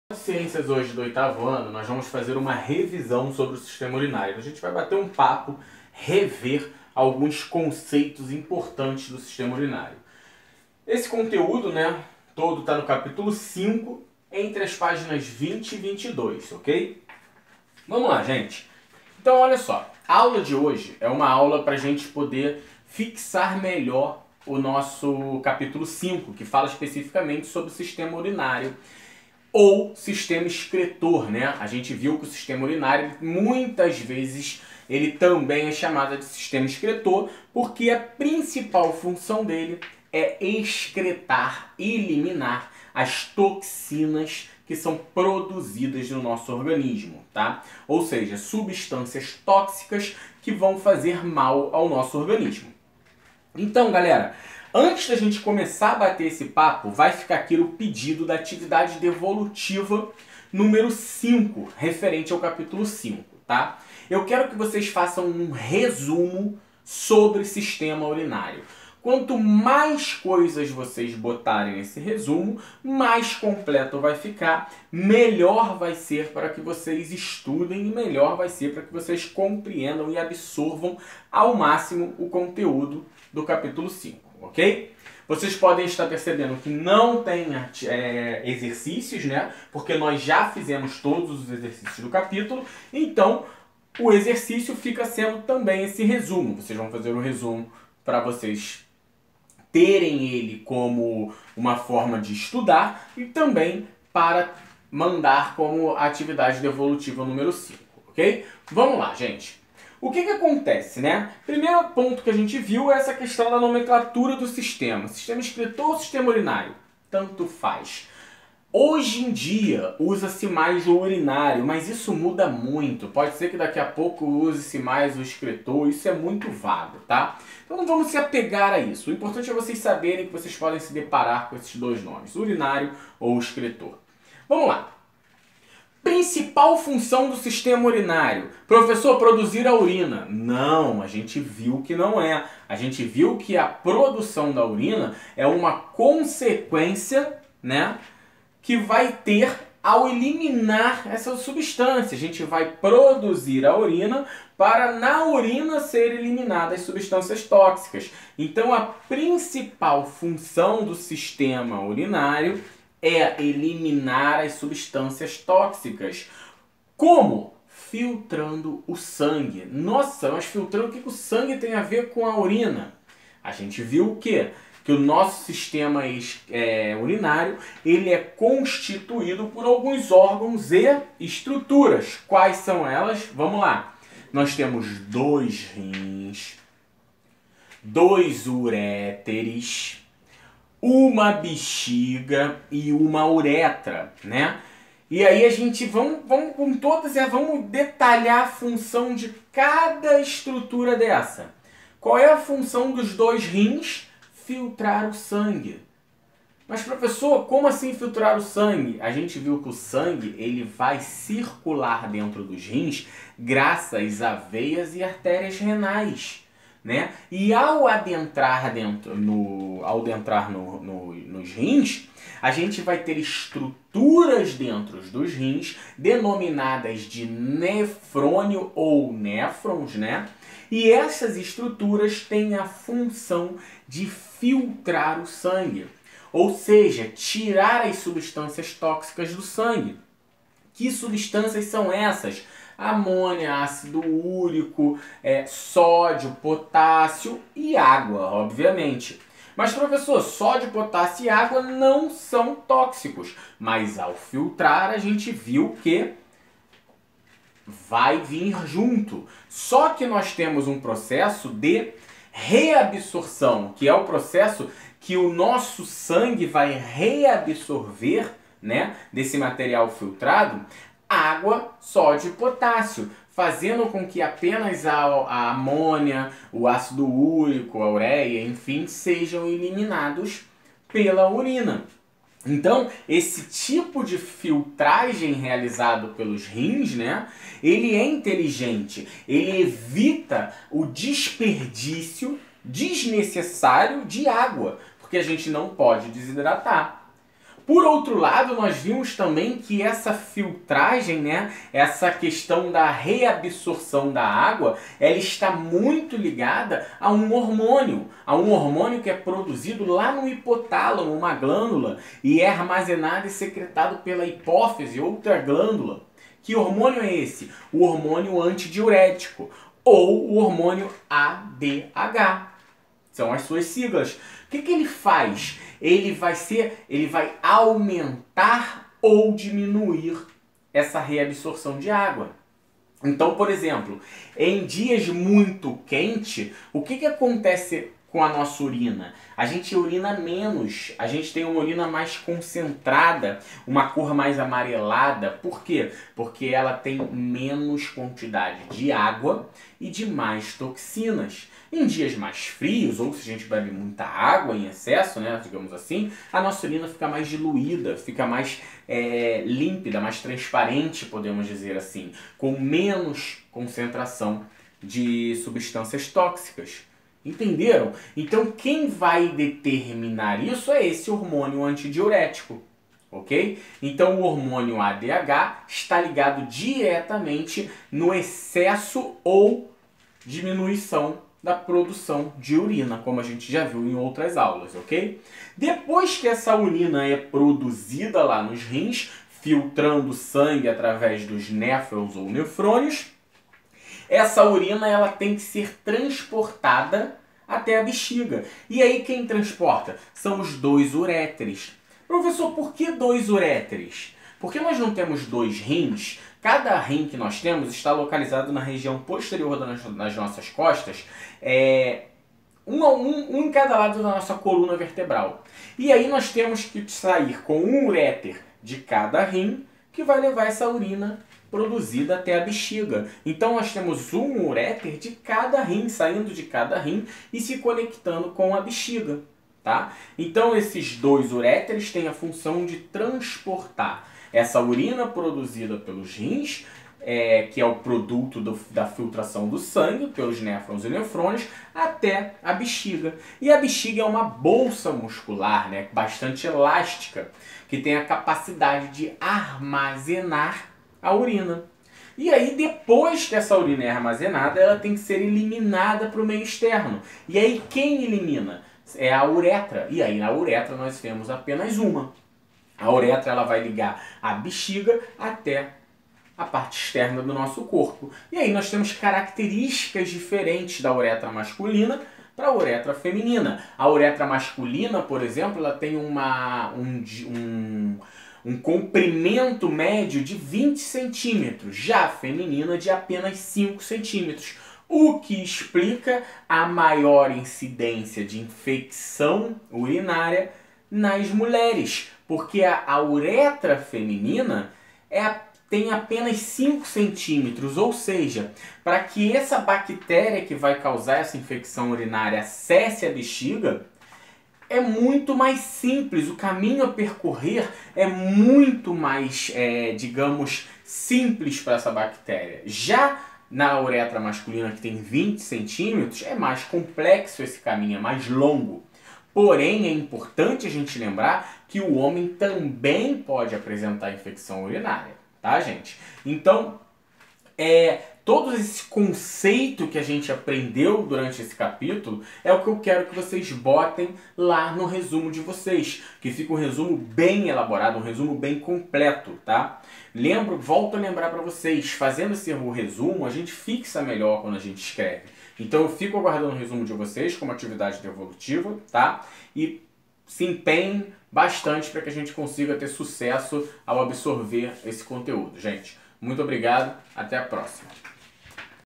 Ciências 8º Ano Ensino Fundamental II (aula V Cap.V - Revisão Sistema urinário + AD 5)
Aula V Cap.V - Revisão Sistema urinário + AD 5
Professor Raphael de Marco